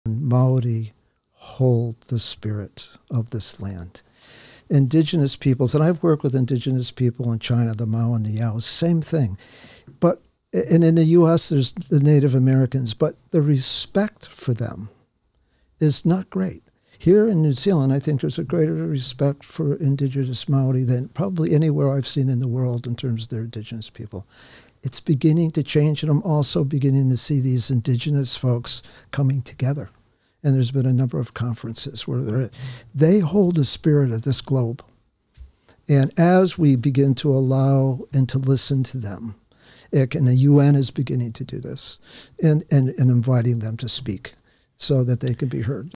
Maori Hold the Spirits of This Land
Does life get so busy sometimes that you even forget to breathe properly? You are not alone! Stephen is a breathwork practitioner who spent much of his adult life studying spiritual psychology with masters from different spiritual traditions. He managed many successful companies in the nutraceutical industry and consults with businesses to bring more effective communications and an understanding of how to create a more effective, sustainable, and profitable business.

Meet Stephen LeFebvre

Stephen is a certified breathwork practitioner with the New Zealand Association of Breathworkers, a personal and business consultant, and a certified breath worker by ISLP in the USA. He has been studying, teaching, and working professionally as a breath worker and consultant since 1989.

Stephen spent much of his adult life studying spiritual psychology with masters from different spiritual traditions, Benedictines, Hindu, Buddhist, and North American Indian. He is a former yoga and meditation teacher, vipassana meditation practitioner, founder of The Southbury Wellness Clinic, nutritionist, men’s group founder, avid gardener, and music lover.

As a past founder and Managing Director of several successful companies in the nutraceutical industry, he also consults with businesses to bring more effective communications and an understanding of how mindfulness contributes to a more effective, sustainable, and profitable business.

ALTBAYS Show invites awesome people to have an honest, laid-back conversation. Our platform focuses on bringing communities together to grow and thrive.